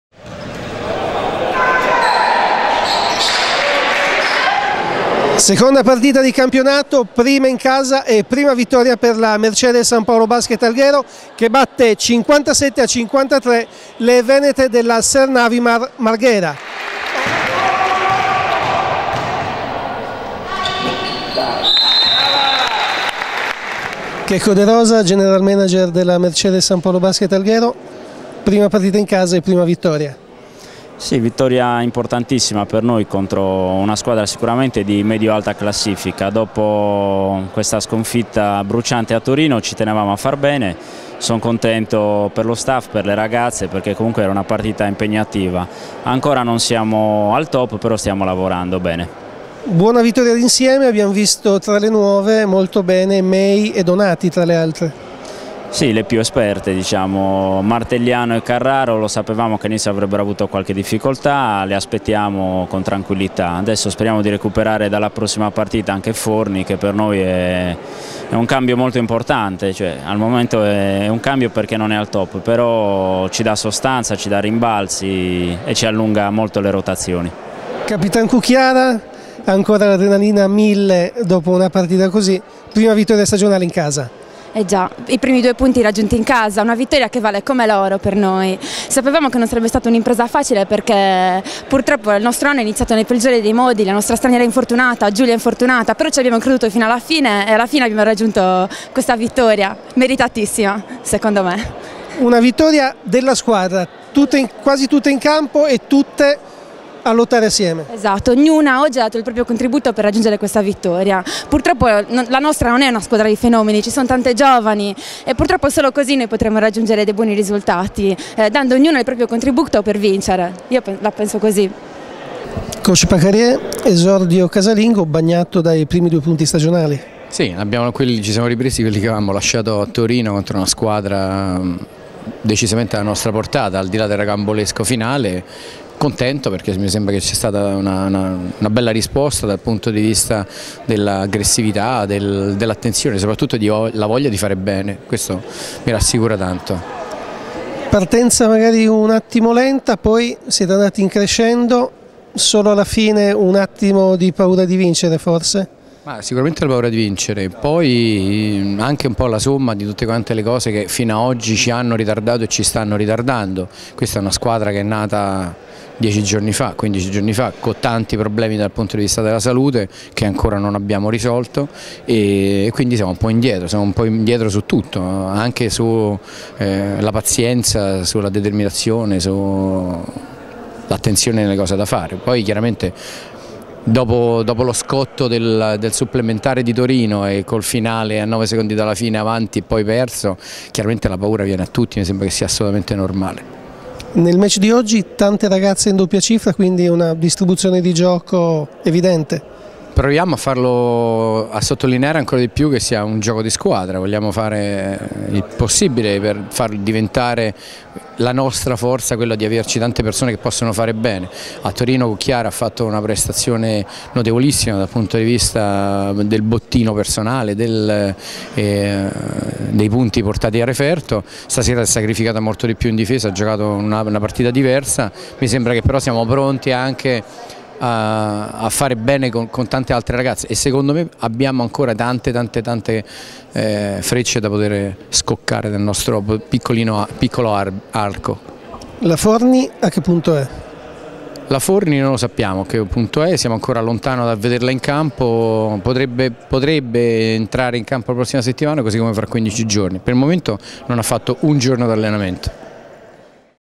Mercede - Marghera 57-53. Intervsta con i protagonisti
Prima vittoria per la Mercede Basket nella A2.